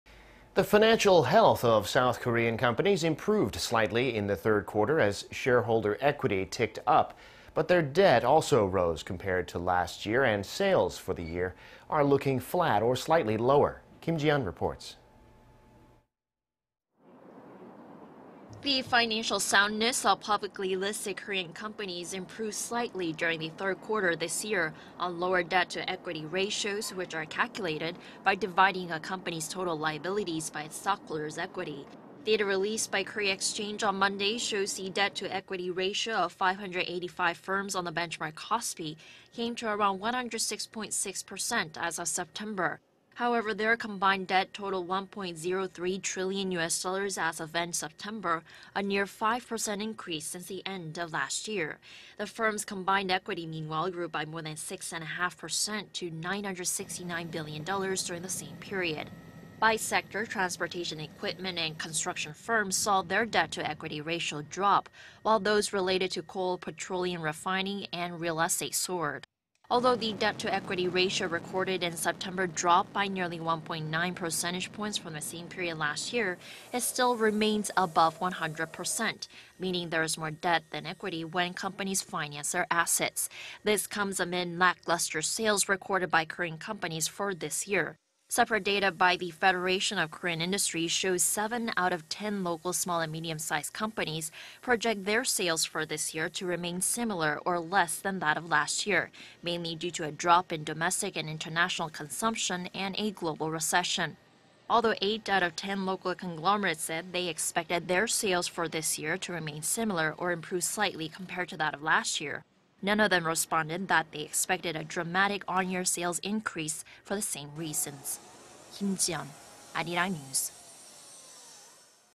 Financial health of listed companies improves, but lackluster yearly sales projected
코스피 상장사 부채비율 소폭 하락, 중소기업 올해 매출 전망 감소예상

The financial health of South Korean companies improved slightly in the third quarter... as shareholder equity ticked up.
But their debt also rose compared to last year,... and sales for this year are looking flat or slightly lower.
Kim Ji-yeon reports.
The financal soundness of publically-listed Korean companies improved slightly during the third quarter this year... on lower debt-to-equity ratios, which are calculated by dividing a company's total liabilities by its stockholders' equity.
Data released by Korea Exchange on Monday shows... the debt-to-equity ratio of 585 firms on the benchmark KOSPI... came to around 106-point-six percent as of September.
However, their combined debt totaled 1-point-zero-3 trillion U.S. dollars as of end-September... a near-five-percent increase since the end of last year.
The firms' combined equity, meanwhile grew by more than six-and-a-half percent to 969-billion dollars during the same period.
By sector, transportation equipment and construction firms saw their debt-to-equity ratio drop,... while those related to coal, petroleum refining and real estate soared.
Although the debt-to-equity ratio recorded in September dropped by nearly one-point-nine-percentage-points from the same period last year... it still remains above 100-percent... meaning there is more debt than equity when companies finance their assets.
This comes amid lackluster sales recorded by Korean companies for this year.
Separate data by the Federation of Korean Industries shows seven out of ten local small-and-medium-sized companies project their sales for this year to remain similar or less than that of last year... mainly due to a drop in domestic and international consumption and a global recession.
Although eight out of ten local conglomerates said they expect their sales for this year to remain similar or improve slightly compared to that of last year... none of them responded that they expected a dramatic on-year sales increase for the same reasons.
Kim Ji-yeon reports.